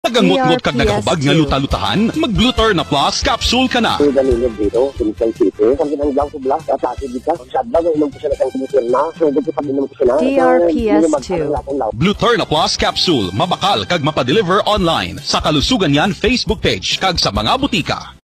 Gluterna Plus Herbal Capsule Radio Ad 2020-2023 18s (Hiligaynon Version/Version 10)
This is the tenth version of the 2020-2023 Radio Ad for Gluterna Plus Herbal Capsule, based in Hiligaynon Language. Mabakal sa mga botika kag farmacia sa iyong lugar.

DRPS Two 4189: Gluterna Plus Herbal Capsule Radio Ad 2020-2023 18s (Hiligaynon Version/Version 10)